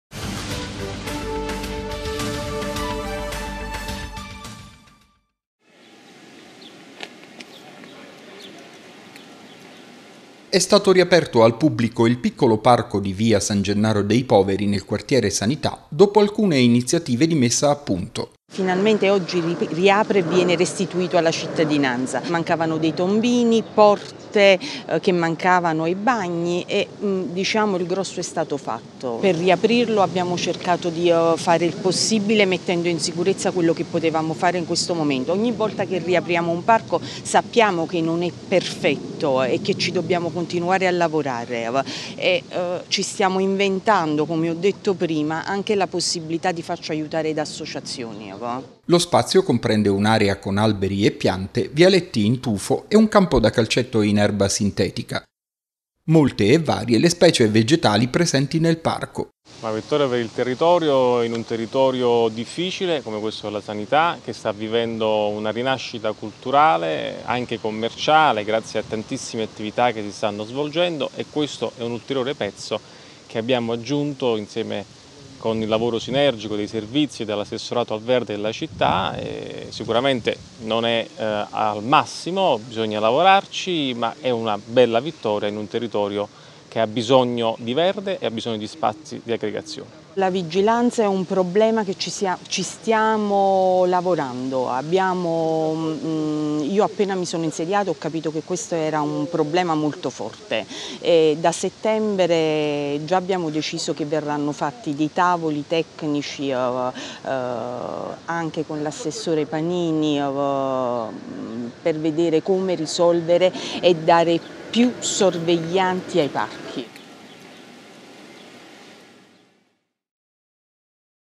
Il parco San Gennaro alla Sanità di nuovo in funzione
Dopo alcuni interventi di messa a punto è stato riaperto il piccolo parco pubblico in via San Gennaro dei Poveri, nel quartiere Sanità.